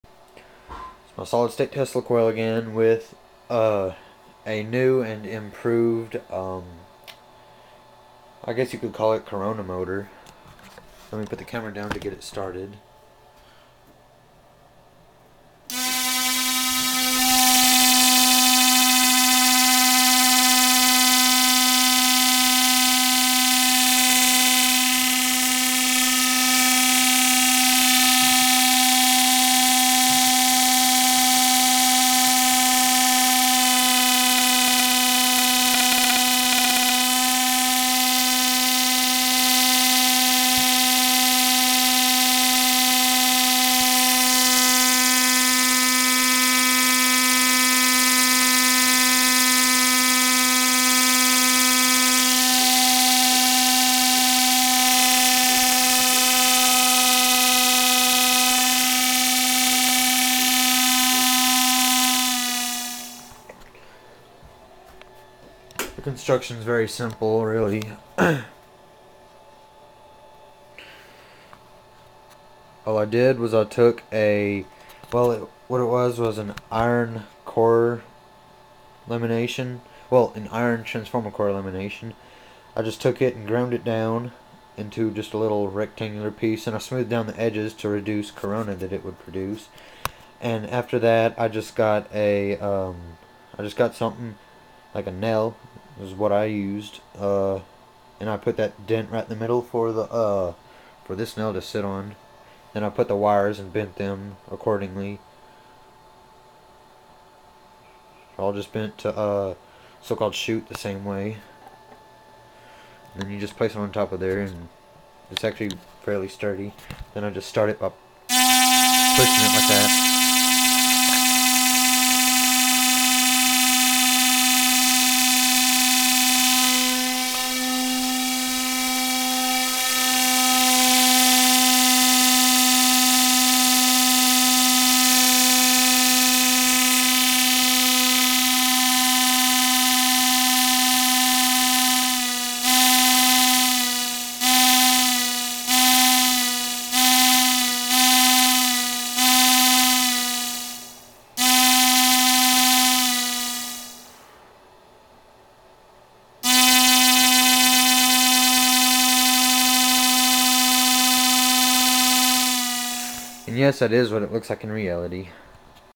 The corona motor.
Using my currently crap PLL driver. It is now being fixed up, so the sparks should be a bit better.

Just a corona motor, very simple in construction. The hardest part is the balance.